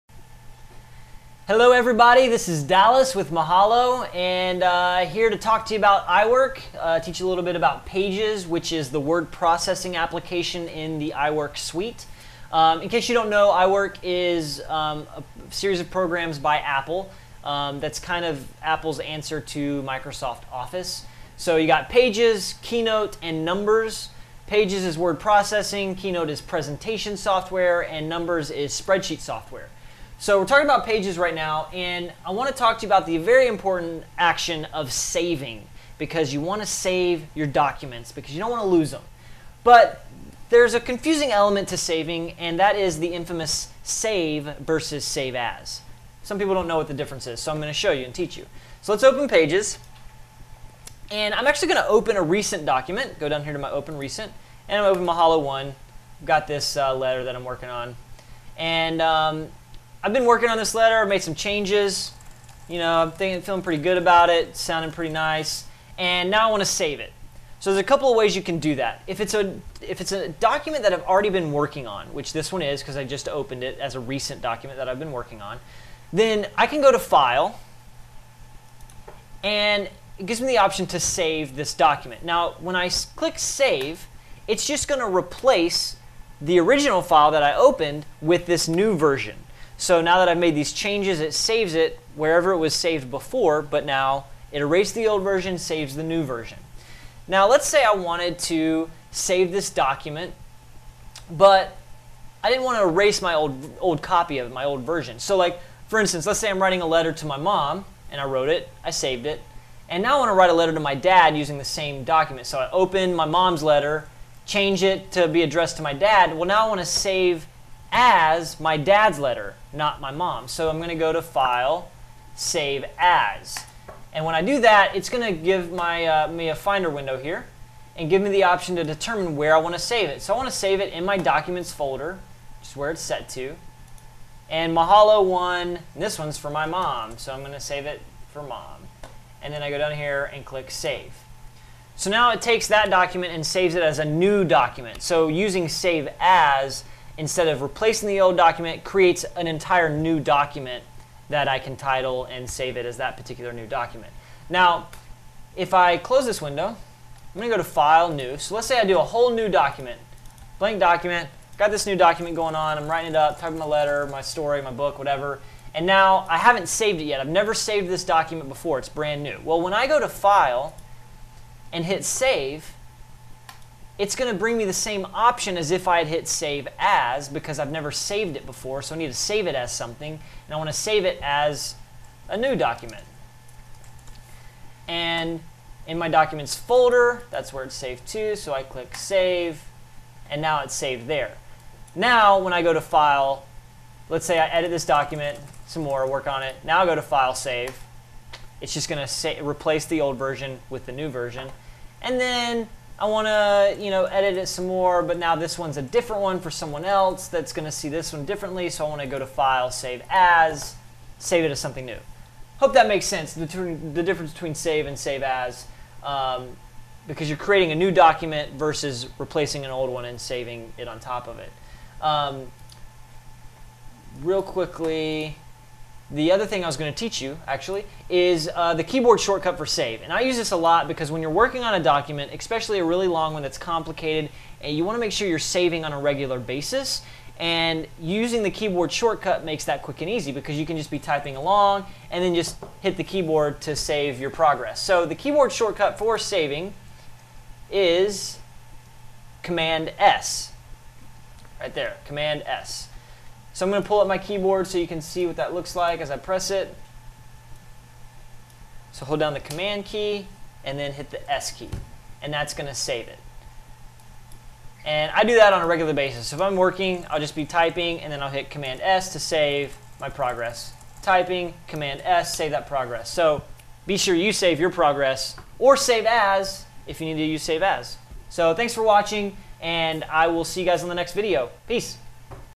How to Save or Save As in Pages
Mahalo's iWork trainer, Dallas Morgan, teaches you how to use different features of Apple's Pages application.